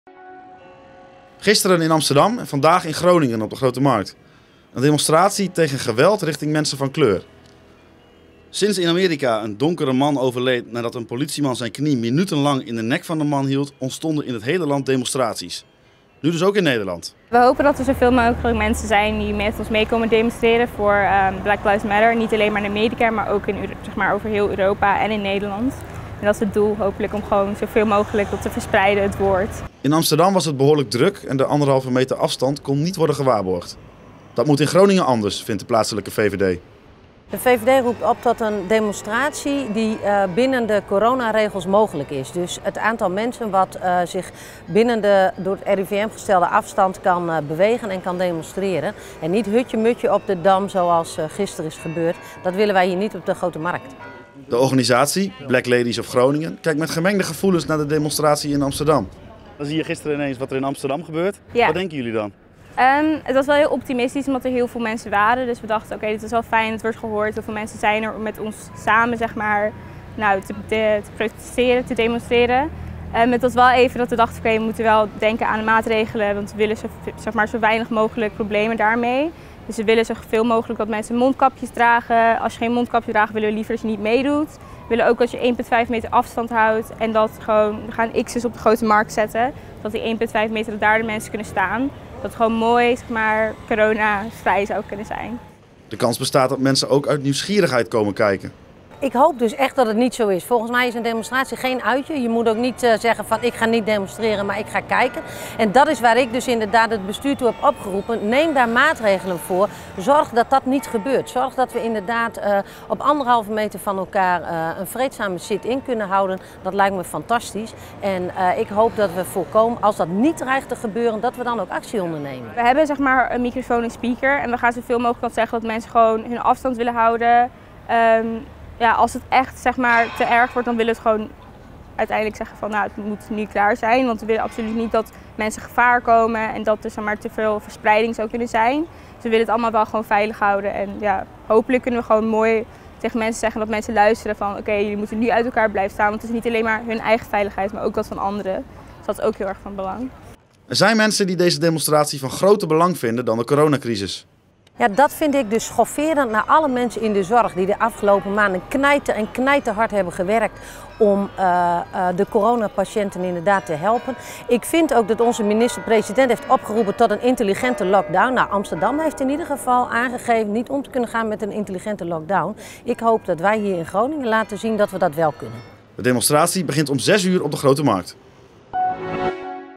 VVD over mogelijke demonstratie tegen racisme: ‘Geen Amsterdam in Groningen’
De gemeenteraadsfractie van de VVD wil dat het college van B&W nadenkt over maatregelen tegen een mogelijke demonstratie tegen racisme en politiegeweld in Groningen. De fractie heeft dinsdag schriftelijk vragen gesteld.

“Hoewel demonstreren een belangrijk grondrecht is, was het op een gegeven moment zó druk dat de maatregelen van het RIVM niet meer nageleefd konden worden. Ook in Groningen wil men vanavond een demonstratie houden”,  zo schrijft de VVD-fractie. “Volgens de berichten op social media worden er in Groningen ruim meer dan 1000 demonstranten verwacht. Een situatie zoals in Amsterdam willen wij in Groningen graag voorkomen.”

De fractie vraagt het college dan ook om na te denken over een alternatieve locatie voor een demonstratie, mocht er reeds een aanvraag voor een demonstratie zijn ingediend bij de gemeente. Ook wil de fractie weten of er gehandhaafd gaat worden op de maatregelen van het RIVM in Groningen.

De gemeente Groningen laat weten in beraad te zijn over hoe met de situatie om te gaan, mocht deze zich voordoen.